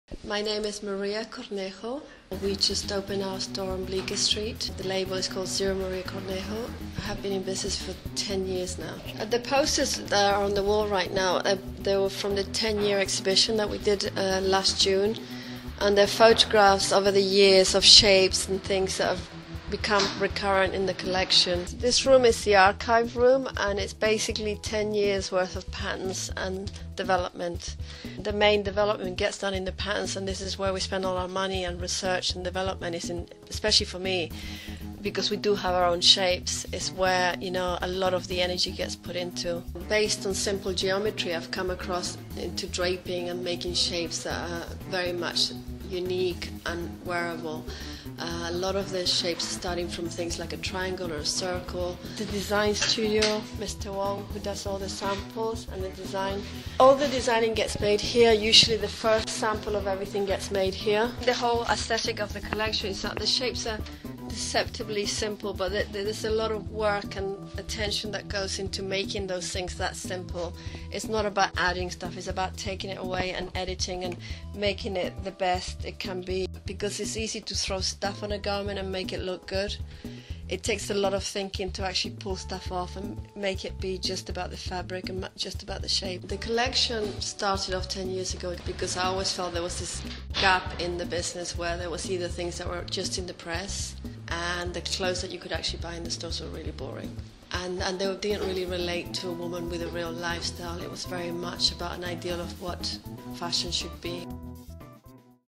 Studio tour: Zero + Maria Cornejo
Walk through Maria Cornejo's Bleecker Street location--which also serves as her shop, studio and showroom--and see why she's a rising fashion tour de force.